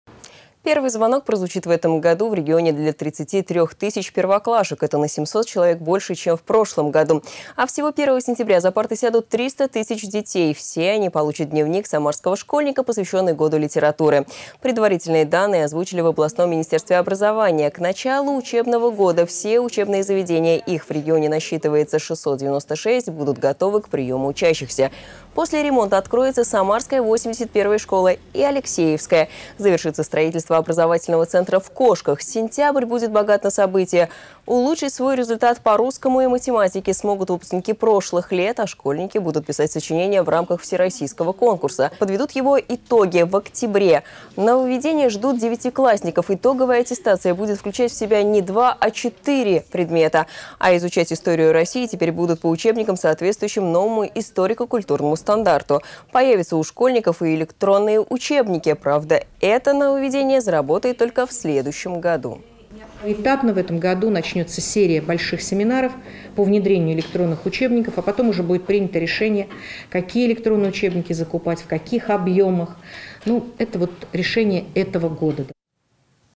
Школьников ждут нововведения с 1 сентября 2015 года
Первый звонок прозвучит в этом году в регионе для 33 тысяч первоклашек. Это на 700 человек больше, чем в прошлом году. А всего 1 сентября за парты сядут 300 тысяч детей. Все они получат дневник самарского школьника, посвященный Году литературы. Предварительные данные озвучили в областном Минобре. К началу учебного года все учебные заведения - их в регионе насчитывается 696 - будут готовы к приему учащихся. После ремонта откроется самарская 81-я и Алексеевская школы. Завершается строительство образовательного центра в Кошках. Сентябрь будет богат на события: улучшить свой результат по русскому и математике смогут выпускники прошлых лет. А школьники будут писать сочинение в рамках всероссийского конкурса. Подведут его итоги в октябре. Нововведения ждут девятиклассников. Итоговая аттестация будет включать в себя не 2, а 4 предмета. А изучать историю России теперь будут по учебникам, соответствующим новому историко-культурному стандарту. Появятся у школьников и электронные учебники. Правда, это нововведение заработает только в следующем году.